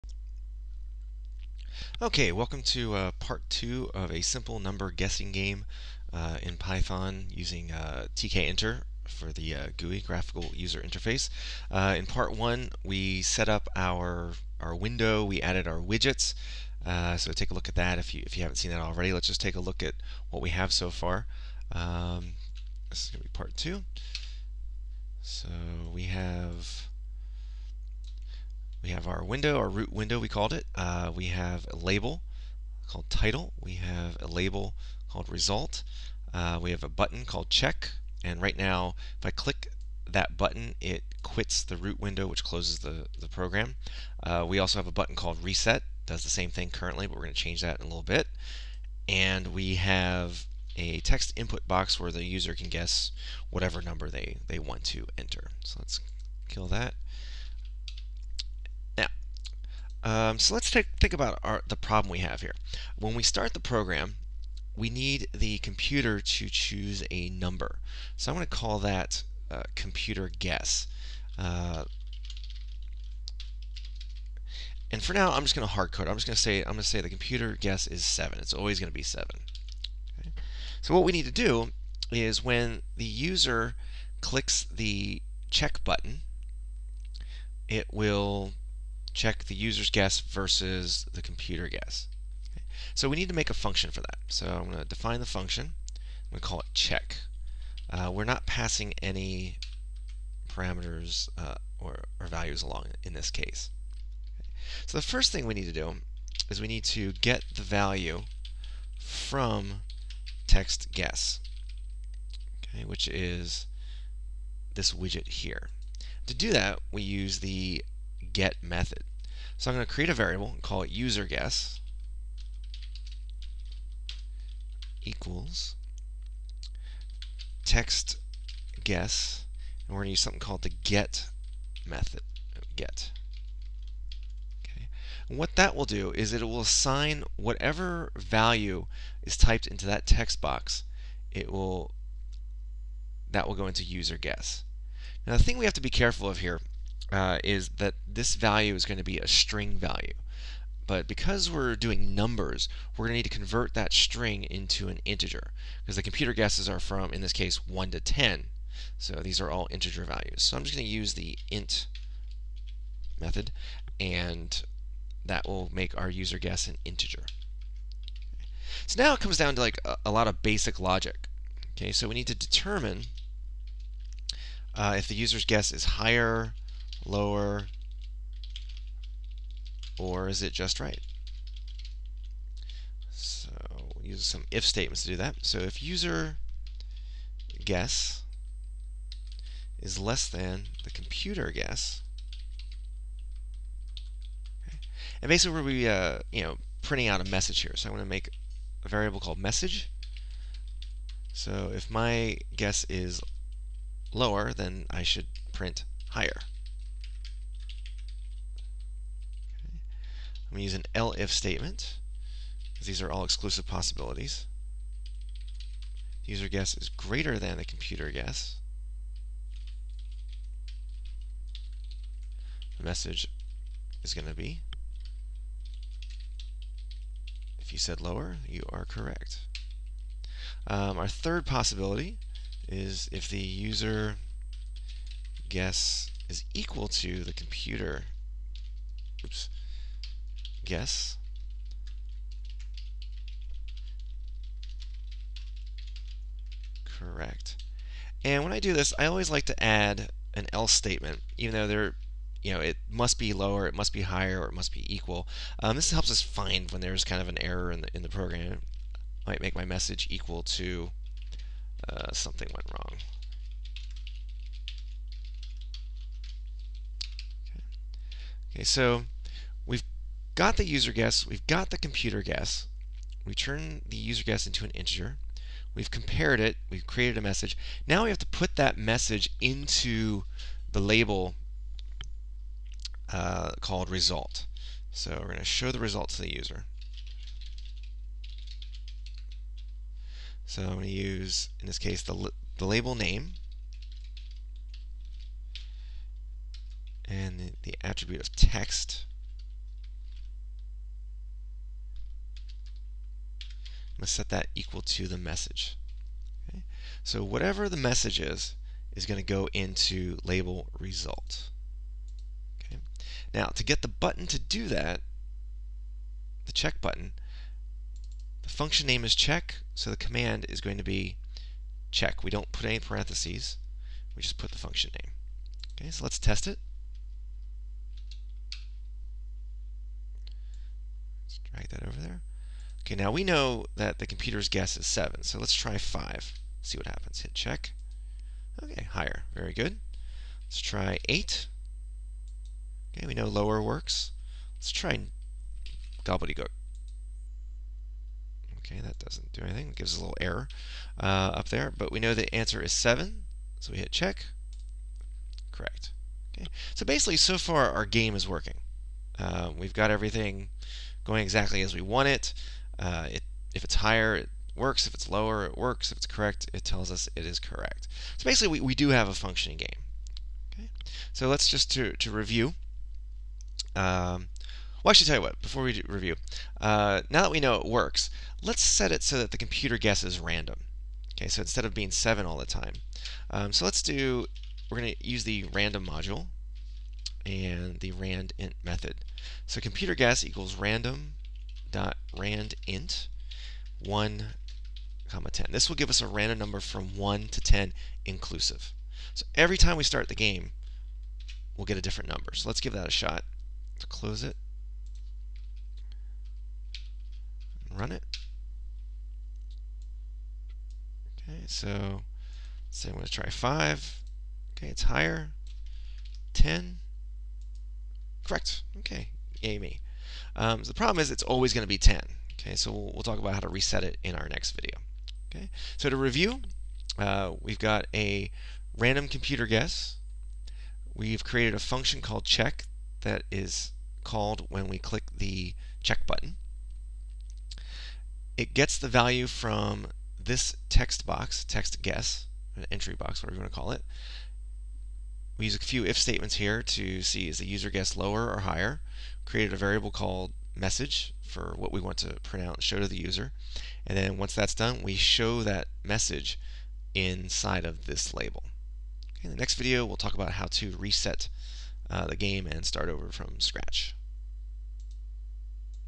Python Guessing Game (GUI Version) Tutorial Pt. 2
How to create a simple GUI Python Guessing Game using Tkinter.  In Part 2, learn how to get the computer to choose a random number, check if the user guess is correct, and inform the user.